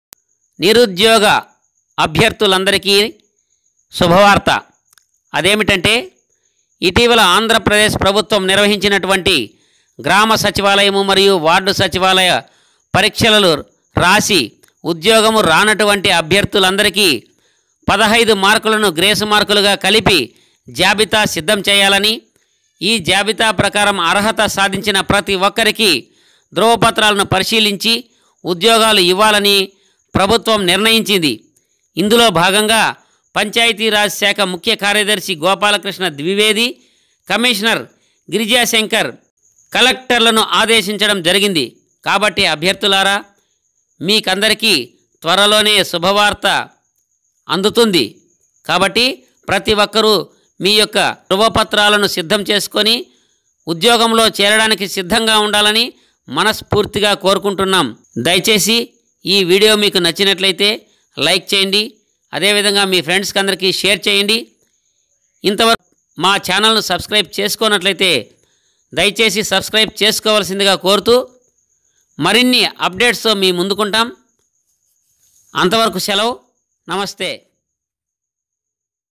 గ్రామసచివాలయం అభ్యర్ధులకు శుభవార్త,ప్రభుత్వ సంచలన నిర్ణయం.....!,ప్రతి అభ్యర్ధి కి 15 marks
ప్రతిఅభ్యర్ధికి15marks కలపనున్నప్రభుత్వంగ్రామసచివాలయం అభ్యర్ధులకు శుభవార్త,ప్రభుత్వ సంచలన నిర్ణయం. !,